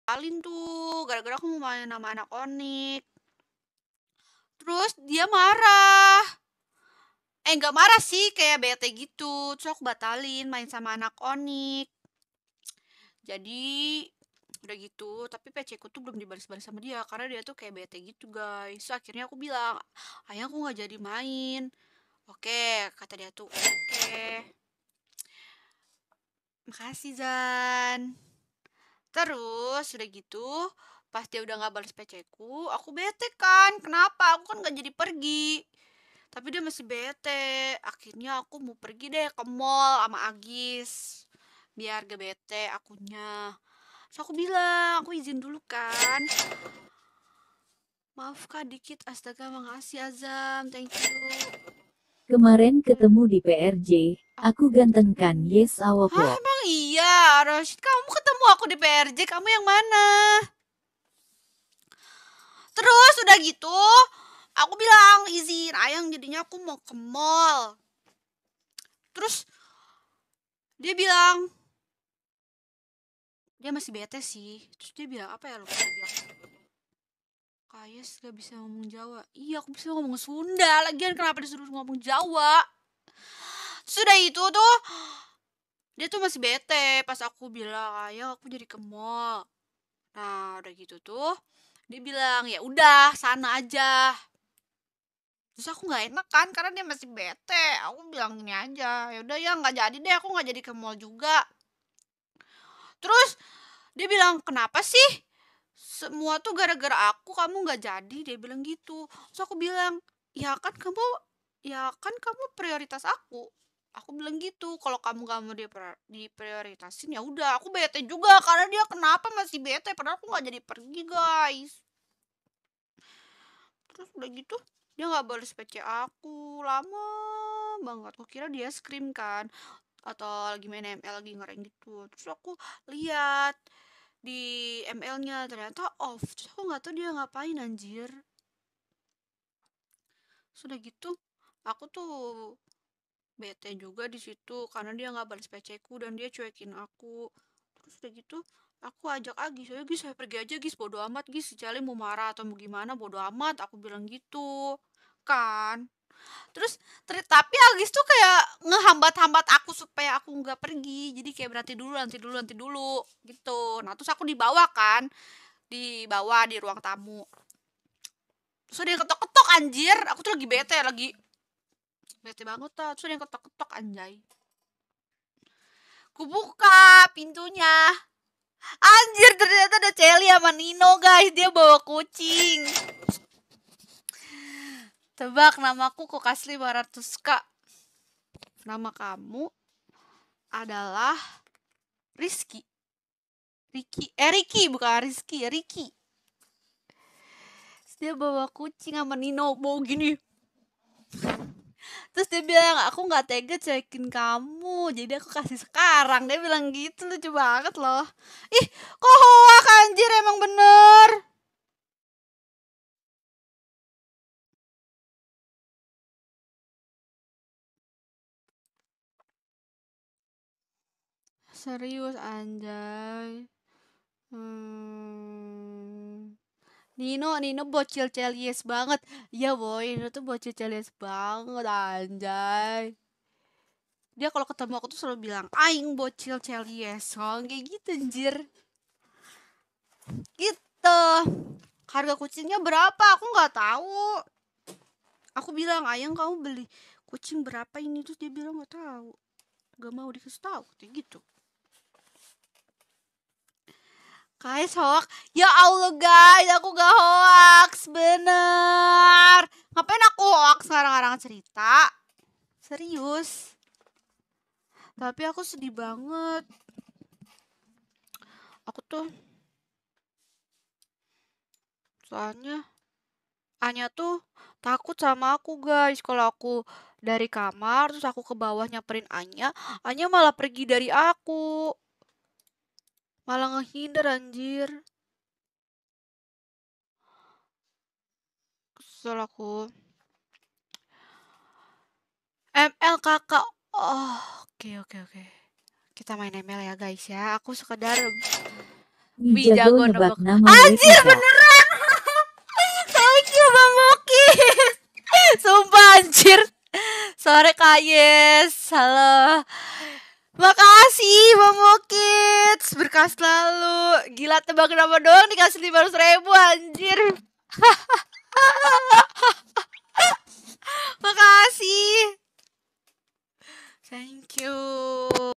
Cerita Seru Kayes! Celiboy Ngambek Dan Surprise Kucing Untuk Kayes
•Thanks

•Thanks•